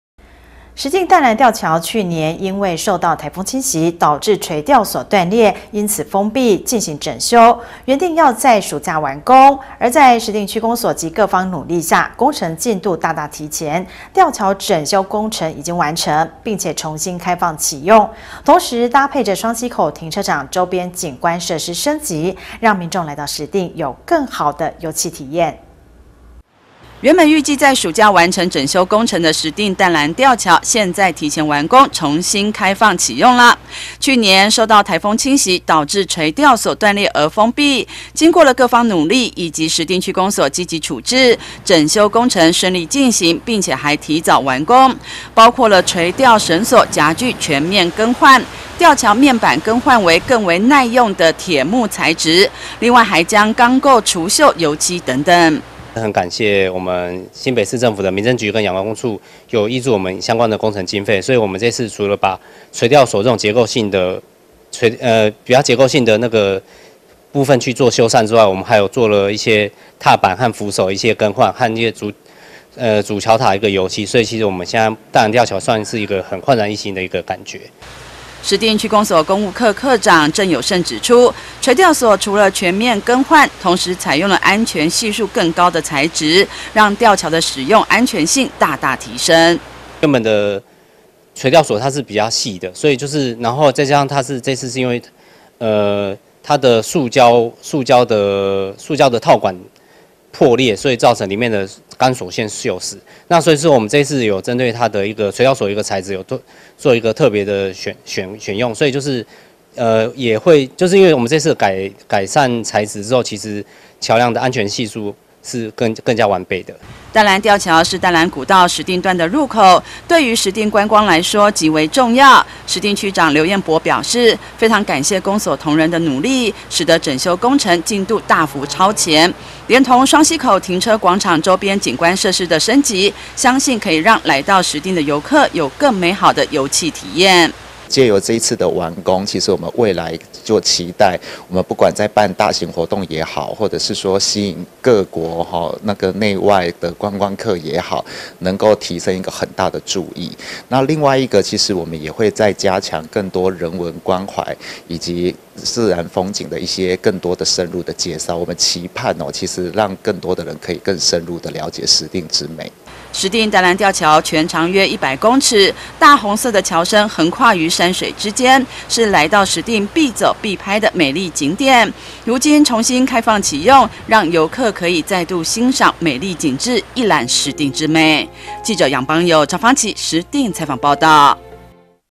石碇淡蘭吊橋整修完工 重新開放啟用
石碇淡蘭吊橋去年因為受到颱風侵襲，導致垂釣索斷裂，因此封閉，進行整修，原定要在暑假完工，在石碇區公所及各方努力下，工程進度大大提前，吊橋整修工程已經完工，並且重新開放啟用，同時搭配著雙溪口停車場周邊景觀設施升級，讓民眾來到石碇有更好地遊憩體驗。